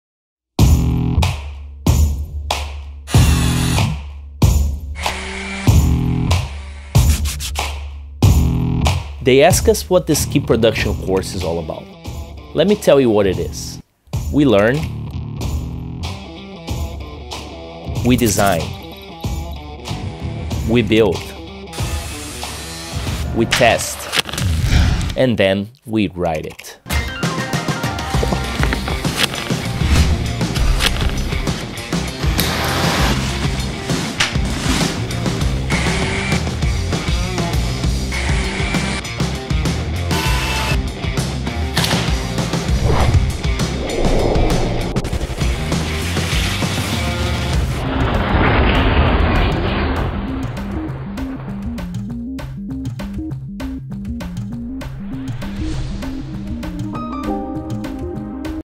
Ski + Snowboard Production | Learn how to design and build your custom ski or snowboard
Ski & Snowboard Production course - part of the Mountain Sport Technician Program.

Whistler Adventure School Career
Mountain Sport Technician.